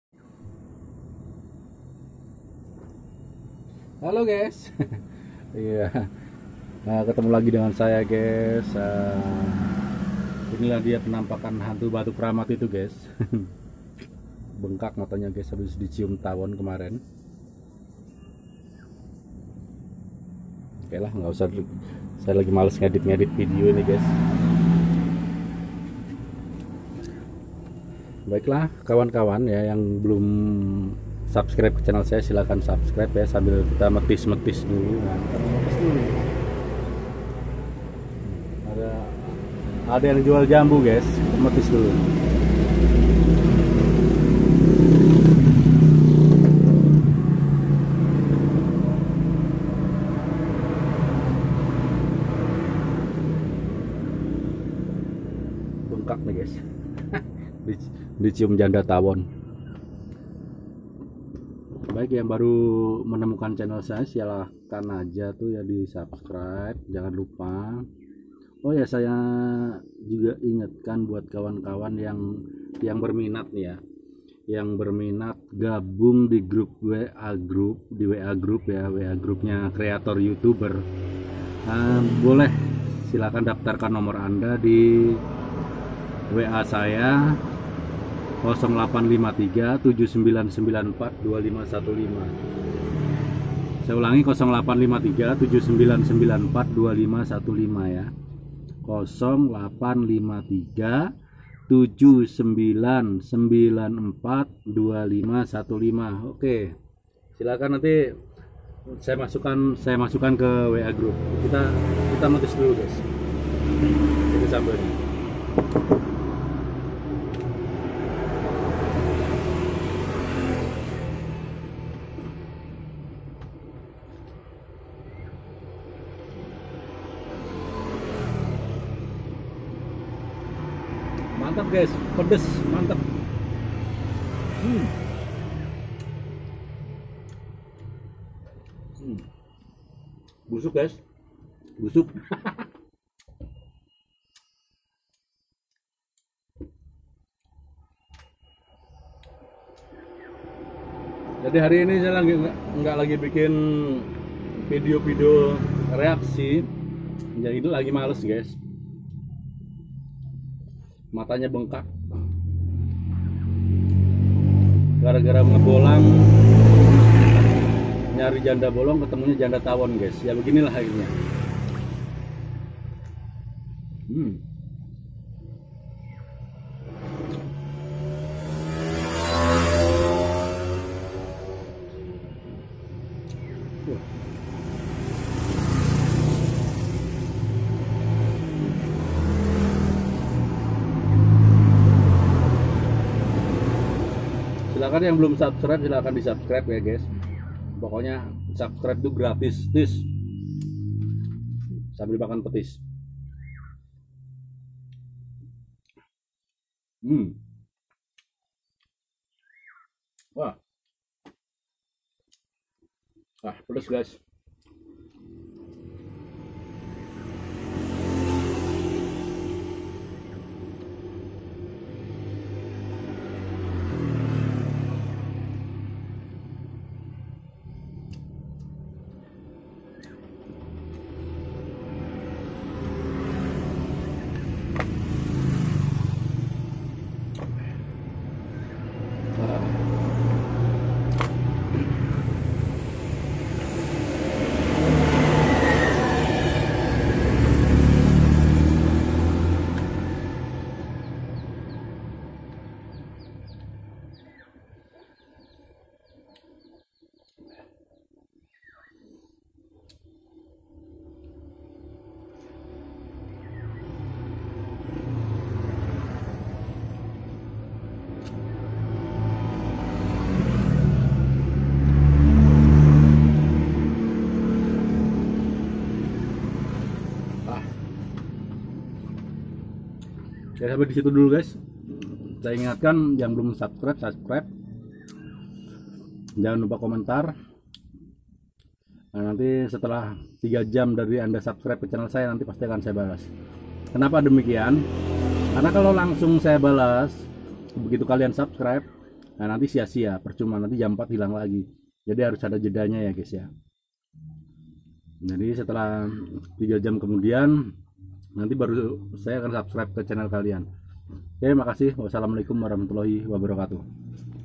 Penjual Buah Yang Cantik // Beli Jambu Bonus Rujakan / Rujak Jambu Puedes Mantaab
Yang berminat gabung di whatsap grup youtuber pemula silahkan chat ke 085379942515